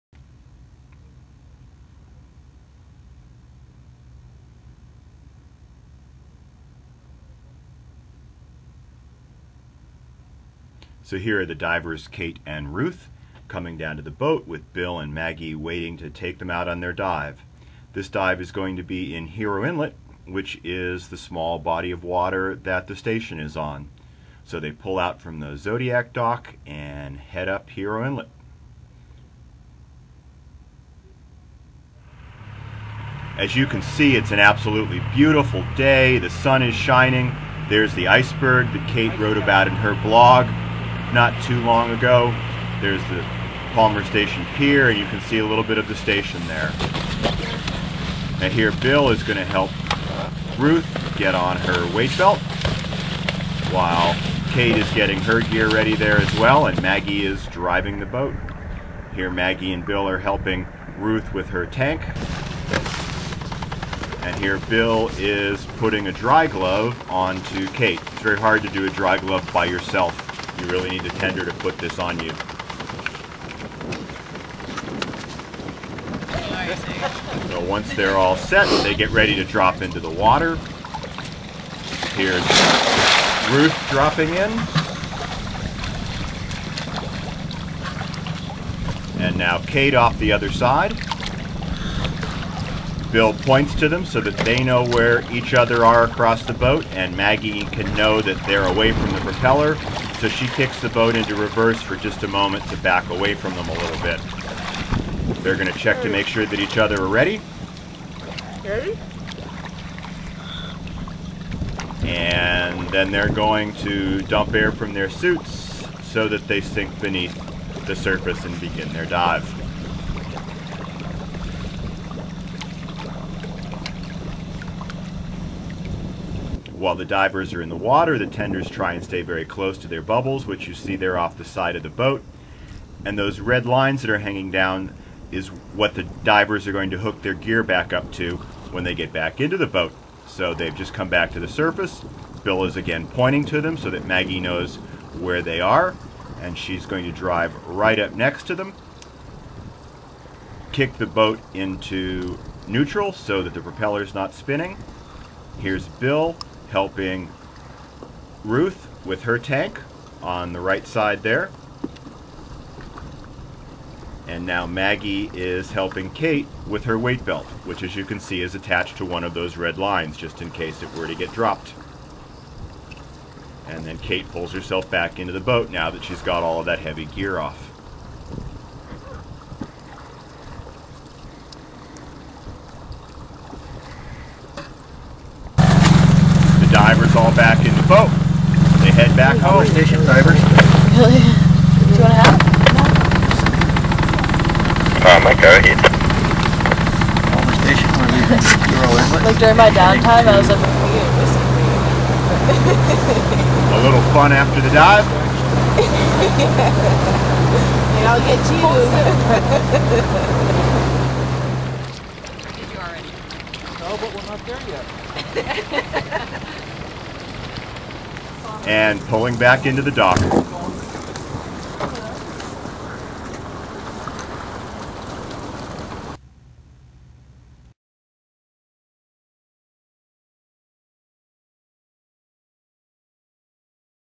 A Dive at Palmer Station, Antarctica
Surface video of Kate Schoenrock and Ruth McDowell making a research SCUBA dive in Hero Inlet, right off Palmer Station in Antarctica. Maggie Amsler and Bill Baker are the dive tenders.